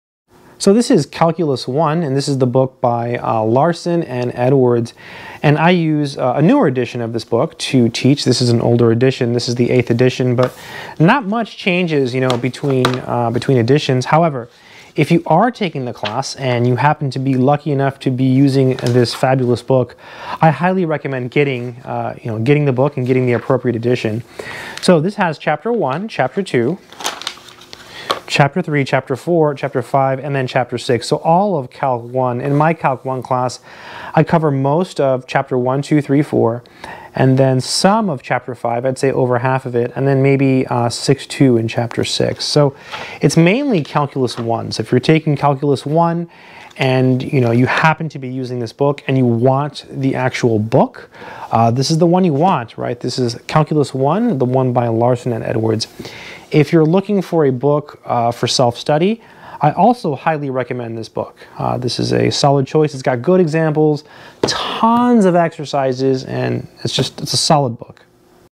Calculus I by Larson
In this video I talk about a very nice book on calculus. This is Calculus I by Larson. I hope this is helpful.

Here is one version that has precalc also